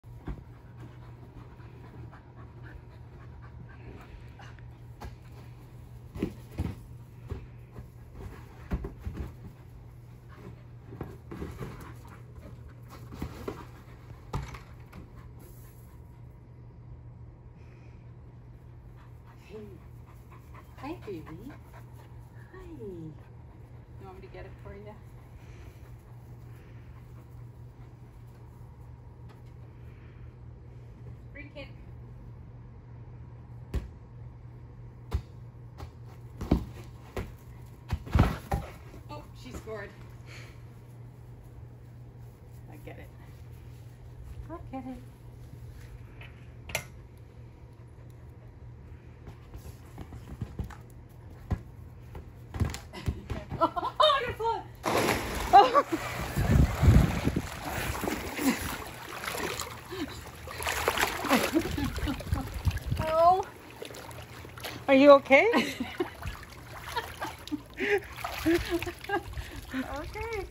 Sheepadoodle, Lady Godiva, Caused This
I was playing soccer with our Sheepadoodle, Lady Godiva,  on an overcast day.  If she got it past me and into the pool it would be a goal!!!  Then this happened.  Wait for it.